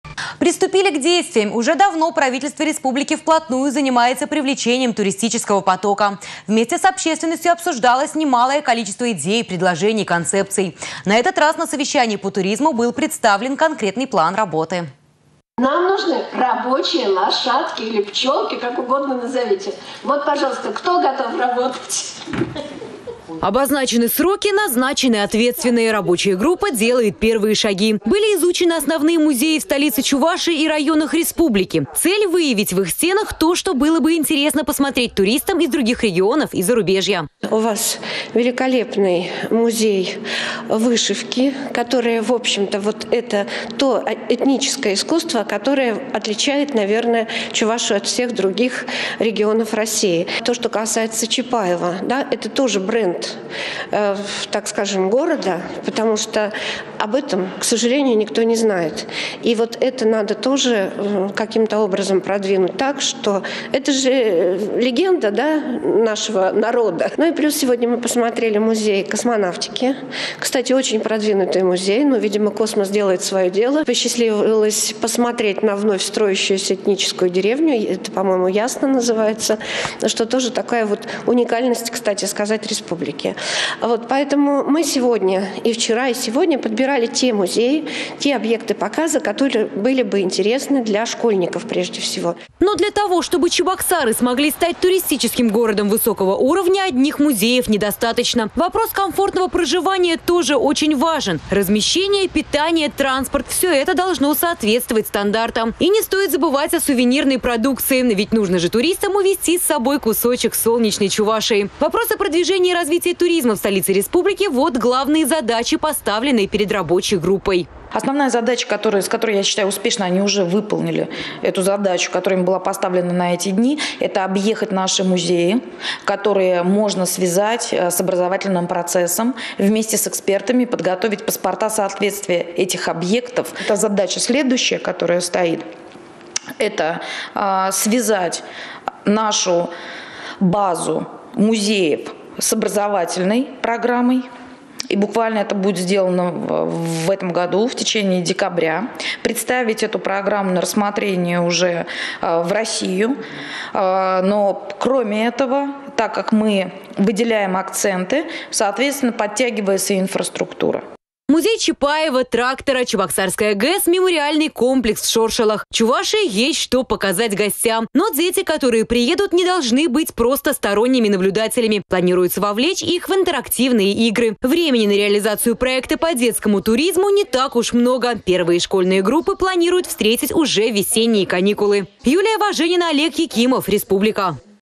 Совещание по туризму
Приступили к действиям. Уже давно правительство  республики  вплотную занимается  привлечением туристического потока. Вместе с  общественностью обсуждалось немалое количество  идей, предложений, концепций. На этот раз на совещании по туризму  был представлен  конкретный план  работы.